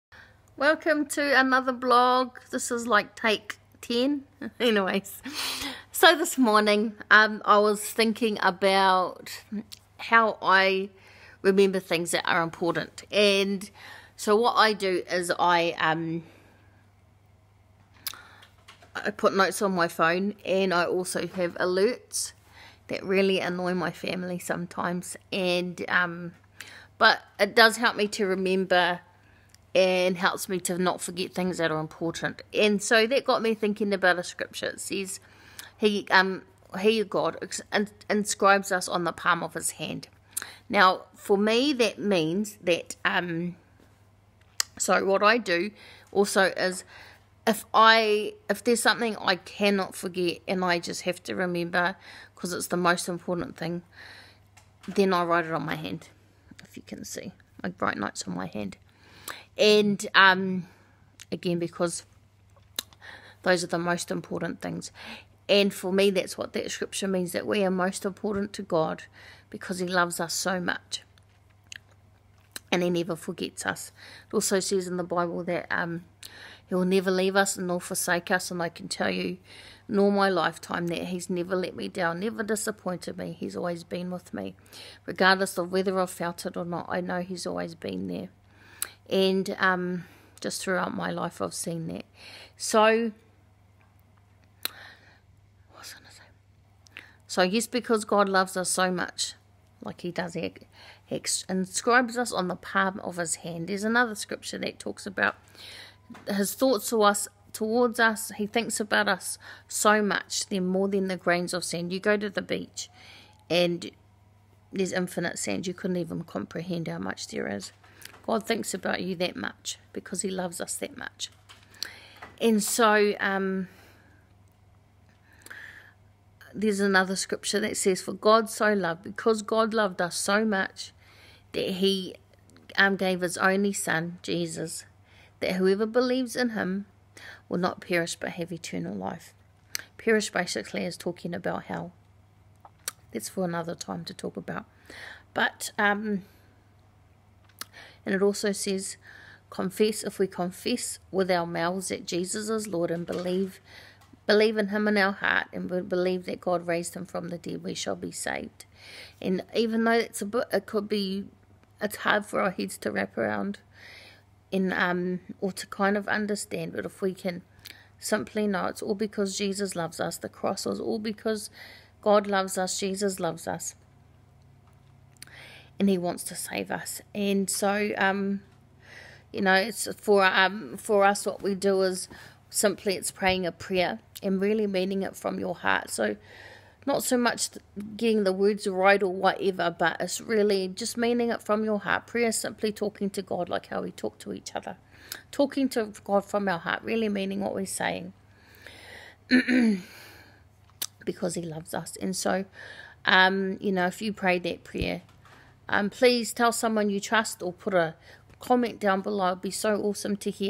YOU ARE MOST IMPORTANT
4/2/19

✝️ JESUS LOVES Y😍U 💟

When I need to remember something important I write up notes or alerts but THE MOST IMPORTANT things I cannot forget,  I write on my hand.
You and I are MOST IMPORTANT to God, our Heavenly Father.

📖 Isaiah 49:16
“See, I have inscribed you on the palms of My hands...”

He never forgets you; He never leaves you; He’s always with you and I 😍

📖 Deuteronomy 31:6-8
“Be strong and of good courage, do not fear nor be afraid of them; for the Lord your God, He is the One who goes WITH YOU. HE WILL NOT LEAVE YOU NOR FORSAKE YOU.”

He is always thinking of you 😍

📖 Psalm 139:17-18
“How PRECIOUS also are Your thoughts to me, O God!
How GREAT IS THE SUM of them! If I should count them, they would be more in number than the sand;
When I awake, I am still with You”

He LOVES you and I so much 😍

📖 John 3:16
For God SO L😍VED THE WORLD that He gave His only begotten Son ✝️ that WH🙂EVER BELIEVES in Him should not perish 🔥 but have everlasting life 😇

So if you want to know your Heavenly Father God Who LOVES you, He’s only a prayer away 💞

📖 Romans 10:9-10
“If you shall CONFESS WITH YOUR
M💞UTH 🙏🏻 the Lord Jesus, and if you shall BELIEVE IN YOUR HEART that God has raised Him from the dead, you shall be saved ✝️

Hope you prayed from your heart, I’m praying for you from my heart.

God bless ya ❤️

P.S. Sorry, my phone memory was full & cut my video short.